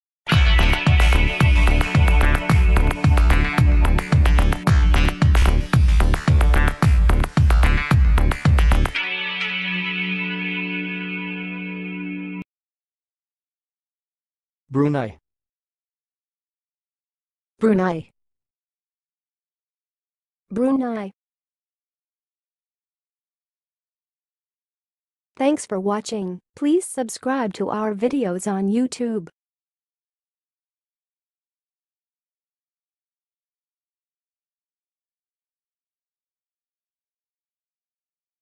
BRUNEI - HOW TO PRONOUNCE IT?
BRUNEI - A guide to pronouncing Brunei with ease in 2023

Brunei, officially known as the Nation of Brunei, the Abode of Peace, is a sovereign state located on the island of Borneo in Southeast Asia.
It is bordered by the South China Sea and Malaysia.
Brunei has a population of approximately 440,000 people and covers an area of about 5,765 square kilometers.
The country is known for its rich oil reserves, with petroleum and natural gas accounting for a significant portion of its economy.
Brunei is governed as an absolute monarchy, with the Sultan of Brunei as the head of state and government.
The nation embraces Islamic culture and is recognized for its high standard of living and healthcare.
The capital city of Brunei is Bandar Seri Begawan, which showcases beautiful mosques and the majestic Sultan Omar Ali Saifuddien Mosque.

Pronunciation:
To pronounce Brunei, you can say 'bruh-ny' or 'broo-n-eye'.

Test yourself by speaking the following examples:
1. Brunei is located on the island of Borneo
2. The Sultanate of Brunei is known for its rich oil reserves
3. Bandar Seri Begawan is the capital city of Brunei